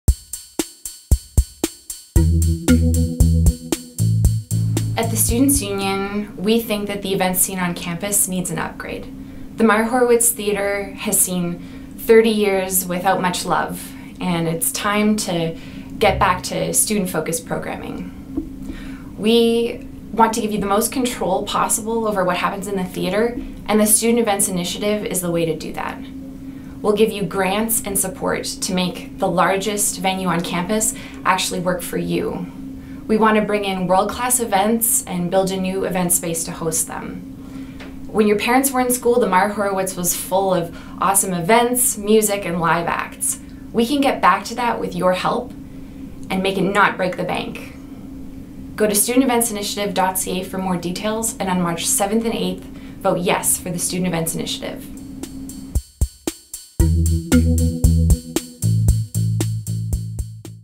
2018 SU Elections: Student Events Initiative Referendum
The Student Events Initiative will be asking students if they support paying $8.25 per student per semester until 2020, and $16.50 per semester after 2020 (the fees are cut in half during spring and summer semesters) to facilitate:

Renovations to the Myer Horowitz Theatre.
The creation of additional event and support space in the Students’ Union Building.
$100,000 in grants for student group events.
$100,000 in funding for featured programming.
An expanded student ticket discount program for events on-campus and in the community.
A commitment from the University of Alberta to invest in renovations to the main floor of SUB to create an accessible north entrance, to appropriately invest in necessary building infrastructure, and, to continue with the student management of common areas through the year 2050.

Video Produced by Rosty Soroka & Oumar Salifou